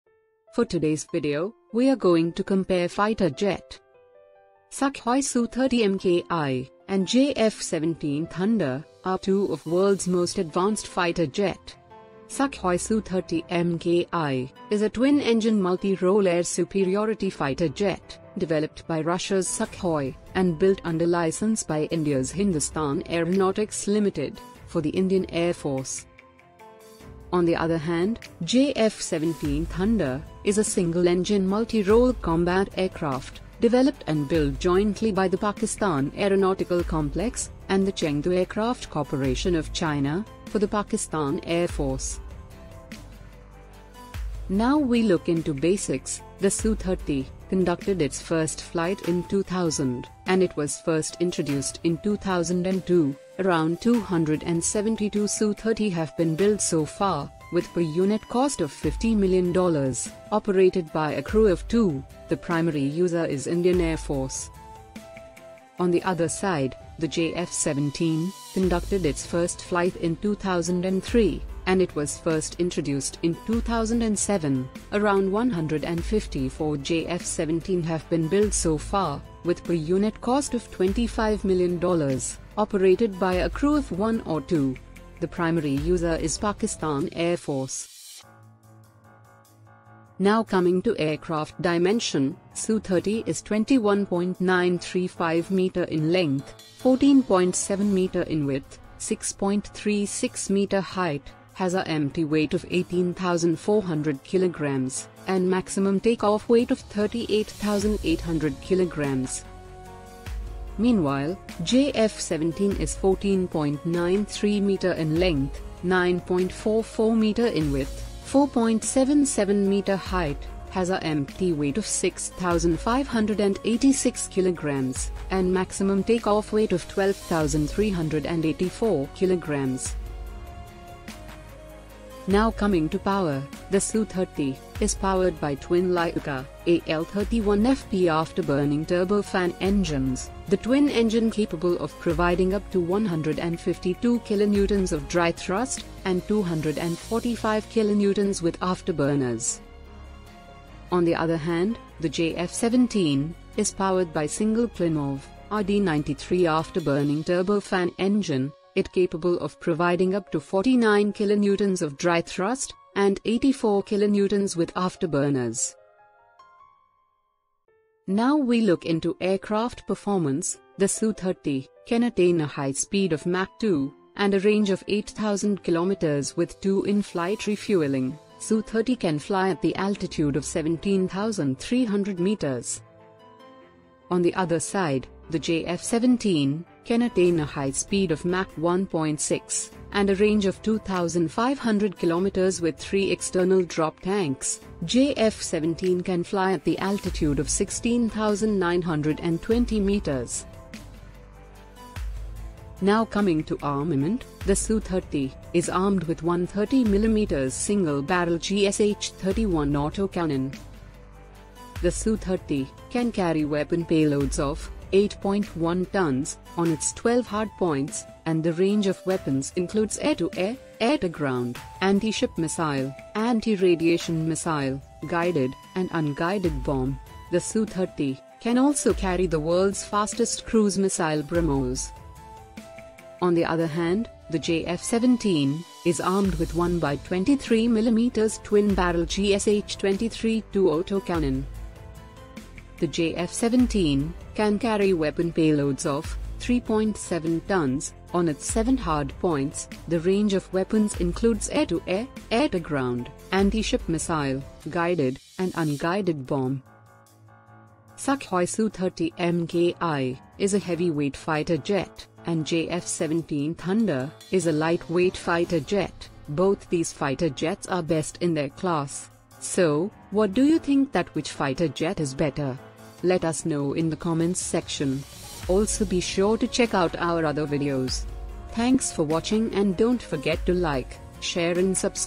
Indian Su-30MKI 🇮🇳 🆚 🇵🇰 Pakistan JF-17 Thunder - Win? | Design of defense system🔥🔥🔥🔥🔥 / dods
The Comparison between Indian Su-30MKI Vs Pakistan JF-17 Thunder.
Sukhoi Su-30MKI and JF-17 Thunder are two of world's most advanced fighter jets.

The Sukhoi Su-30M K I is a twin engine multirole air superiority fighter jet developed by Russia's Sukhoi, and built under licence by India's Hindustan Aeronautics Limited for the Indian Air Force.

on the other hand, The Jf-17 thunder is a single-engine, multi-role combat aircraft developed and build jointly by the Pakistan Aeronautical Complex and the Chengdu Aircraft Corporation of China, for the Pakistan air force.

The Sukhoi Su-30 Mki is a heavy weight fighter jet and Jf-17 thunder is a light weight fighter jet, both these fighter jets are best in their class.
So, what do you think that which fighter jet is better?

This video is only for educational and informational purposes.